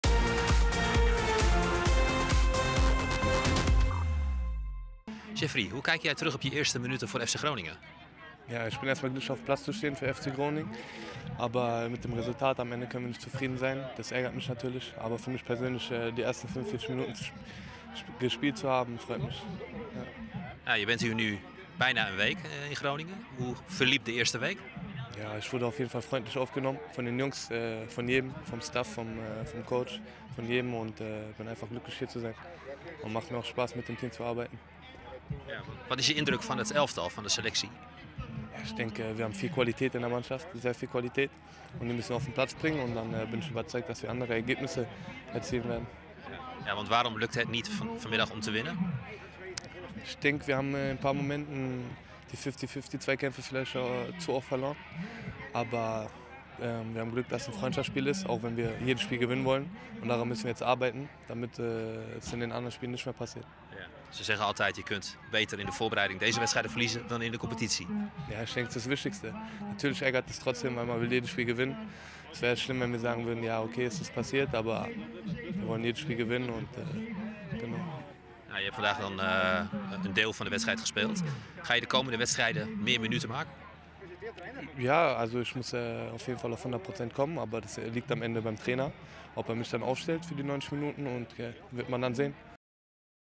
Jeffrey Chabot: "Blij met mijn eerste minuten"
Jeffrey Chabot maakte tijdens de oefenwedstrijd tegen FC Emmen zijn officieuze debuut voor FC Groningen. Hij speelde in Rolde de eerste helft als centrale verdediger.